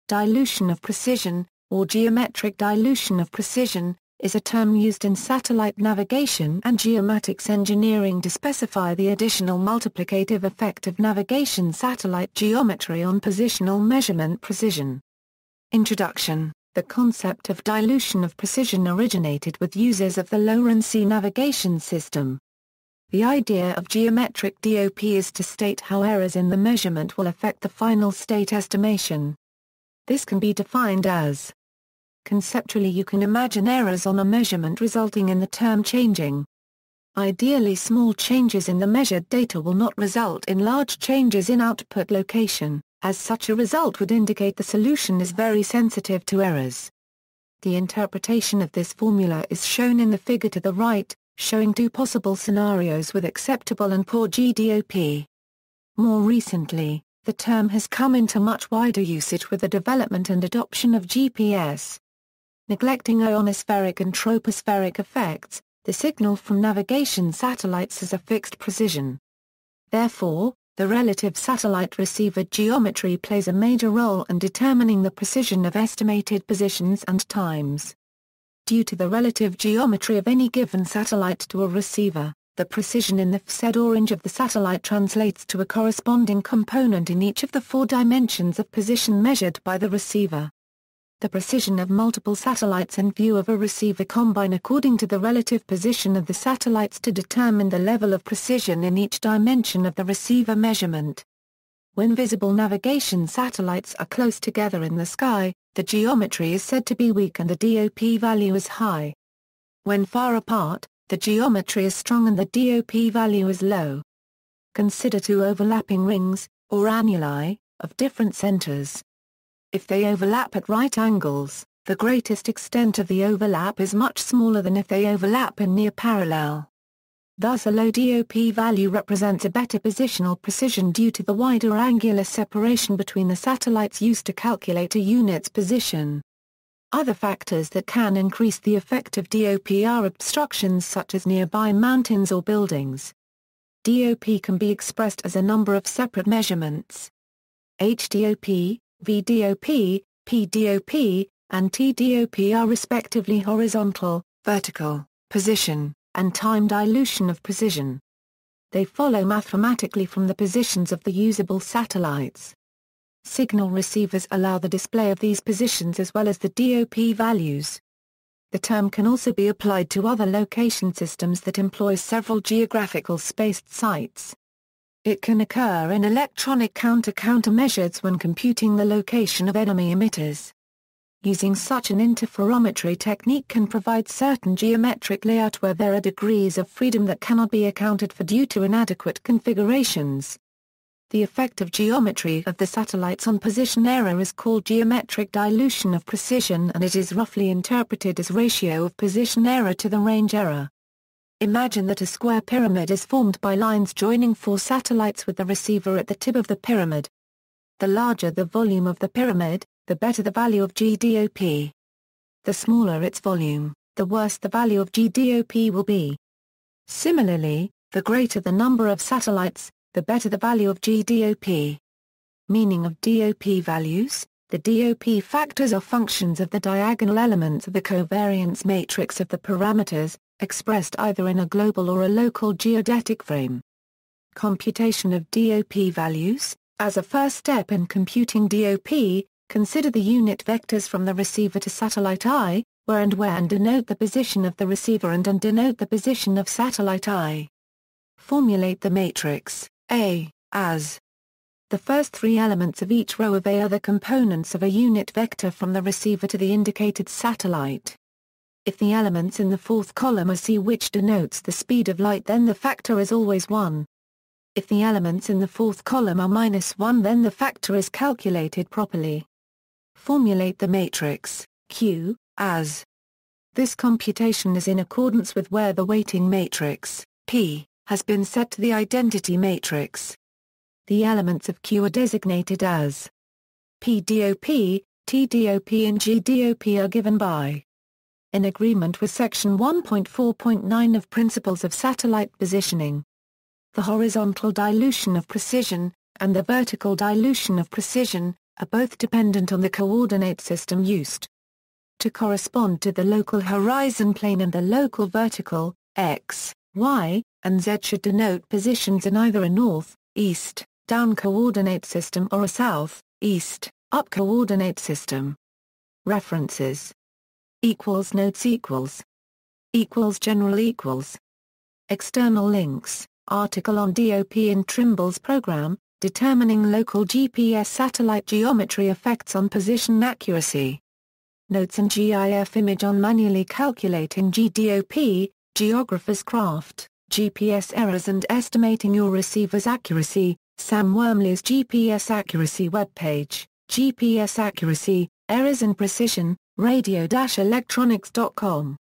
Dilution of precision (GPS)
Dilution of precision, or geometric dilution of precision, is a term used in satellite navigation and geomatics engineering to specify the additional multiplicative effect of navigation satellite geometry on positional measurement precision.